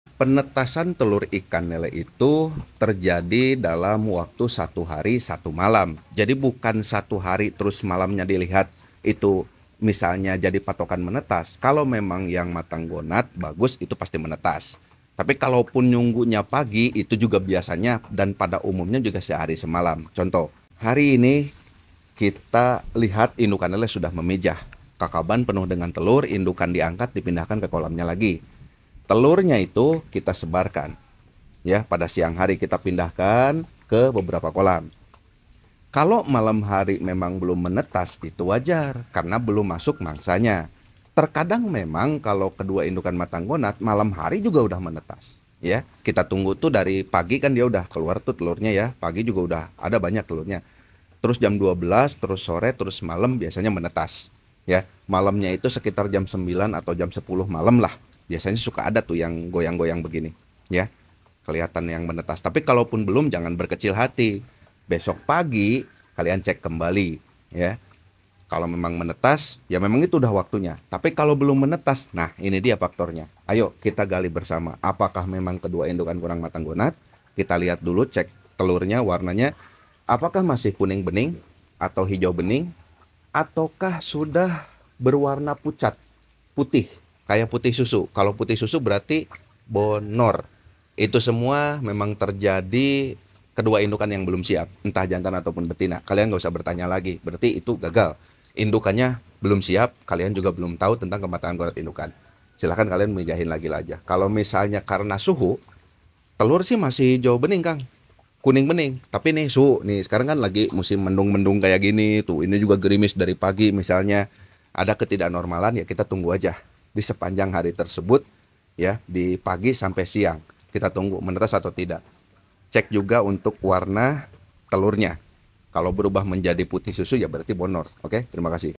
Penetasan telur lele yang IDEALnya itu satu hari satu malam, Bagaimana kalau lebih❓
Penetasan telur lele yang IDEALnya itu satu hari satu malam, Bagaimana kalau lebih❓Mungkin induk lele belum siap kawin atau suhu cuaca kurang mendukung